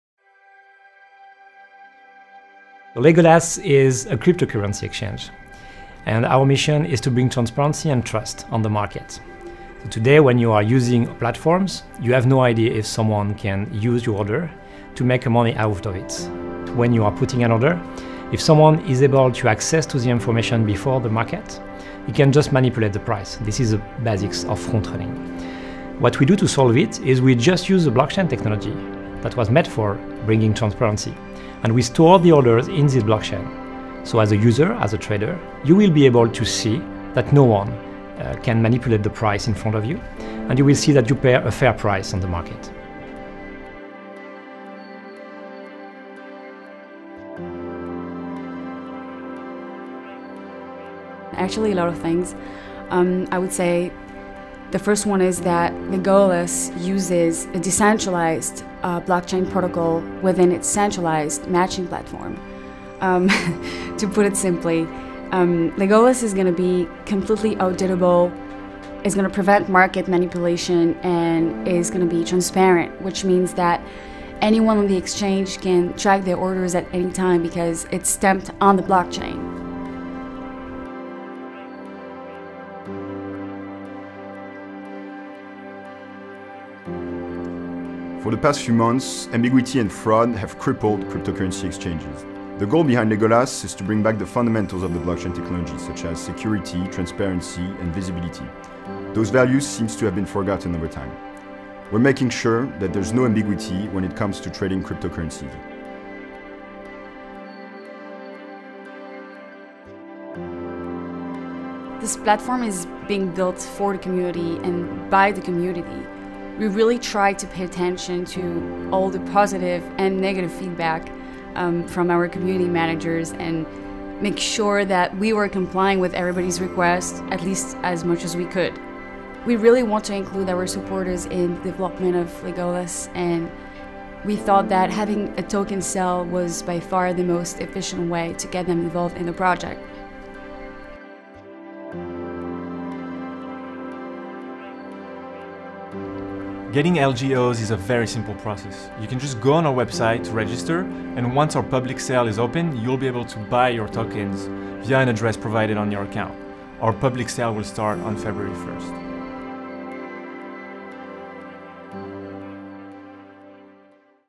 LGO, Fair & Secure by Design
LGO is a cryptocurrency exchange fair & secure by design. We're creating a new decentralized blockchain protocol that guarantees full transparency and prevents front-running and market manipulation.